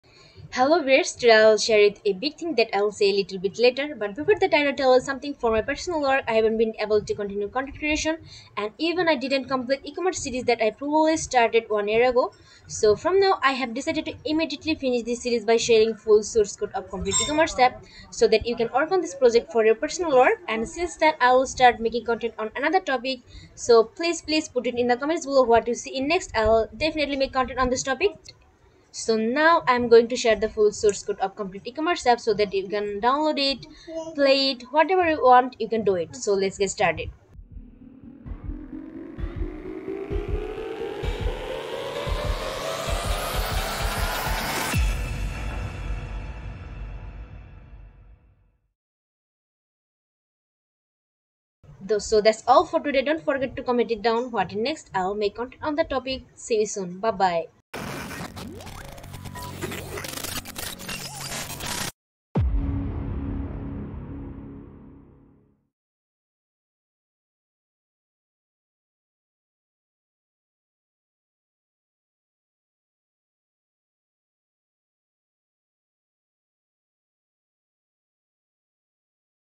Flutter tutorial - Complete Ecommerce App Full Source Code
Today I'm going to share the Ecommerce App's full source code so that you can work on this project for your
own project. And share this with others so that they can also work on this project. Thank you for your support.